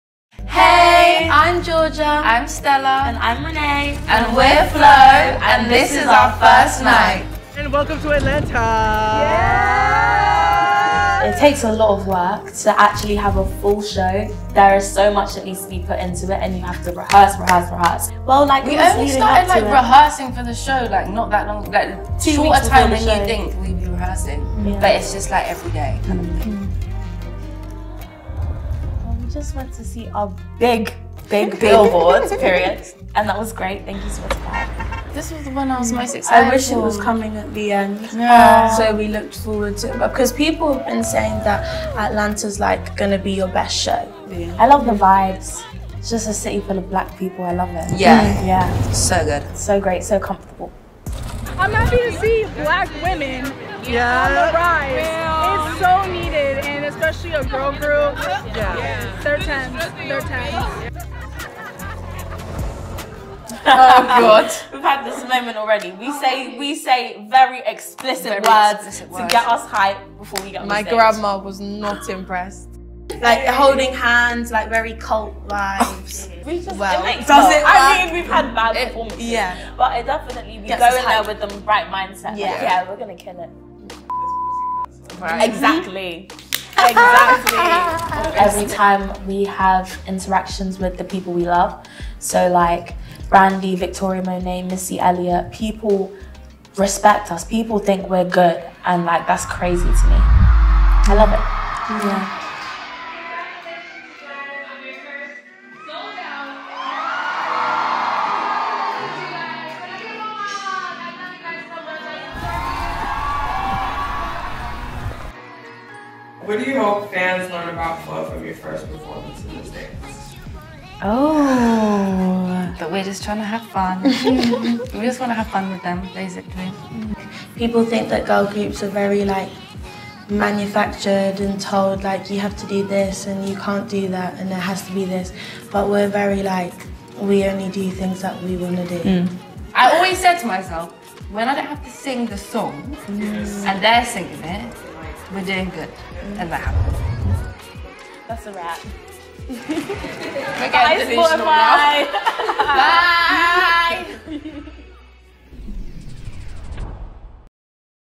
FLO Live with Spotify’s RnBFirstNights
Our first headlining show in ATL was sooo special 🫶🏾 We linked up with Spotify to give you a look at how our sold out concert came together